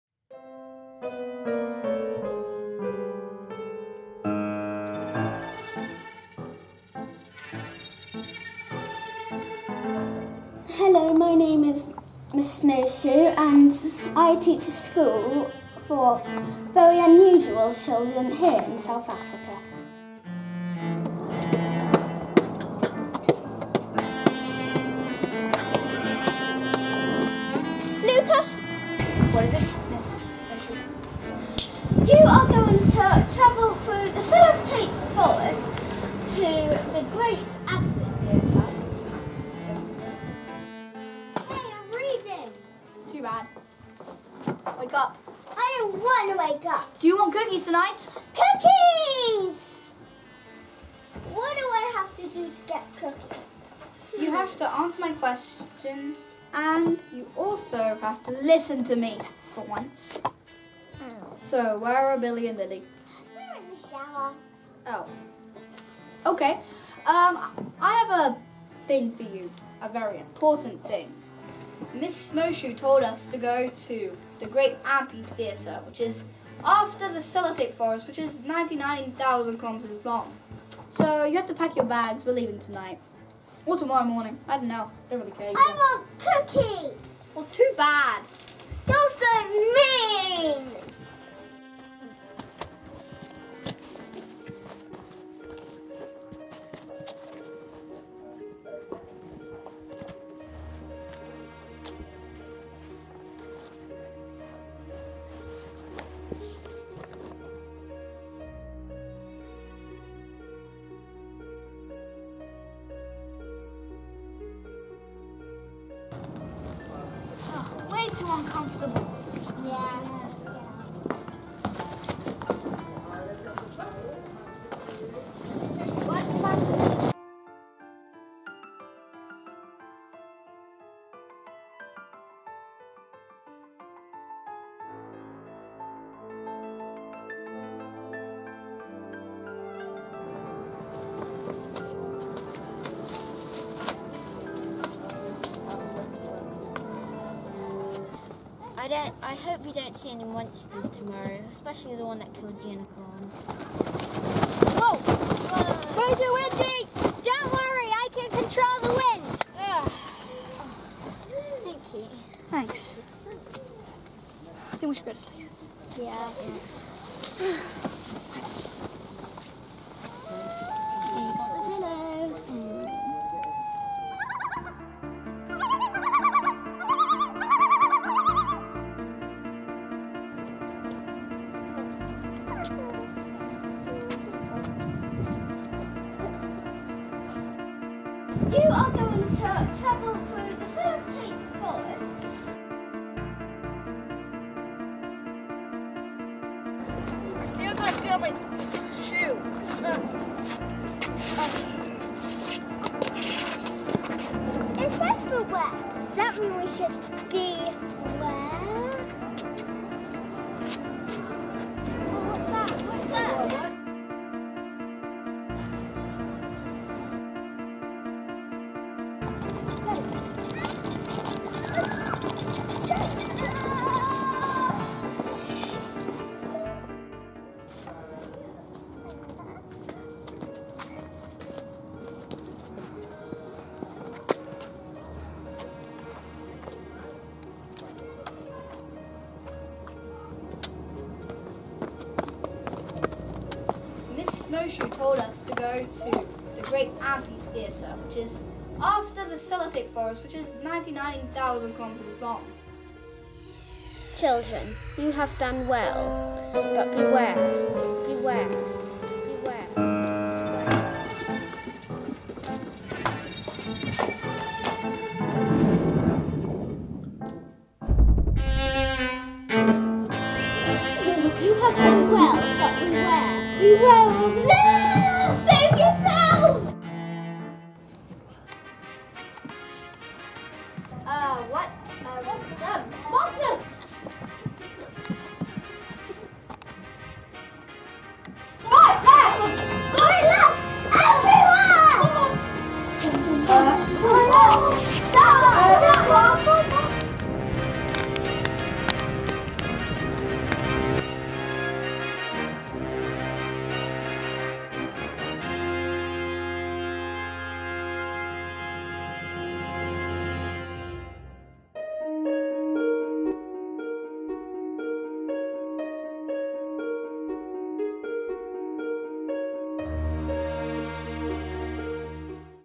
Miss Snowshoe's Challenging Challenge for Unusual Children
A film produced by our Studio 1 group in Balham from the Spring term's Saturday Film Shoots.

For more information on our Saturday workshops, holiday courses and birthday parties please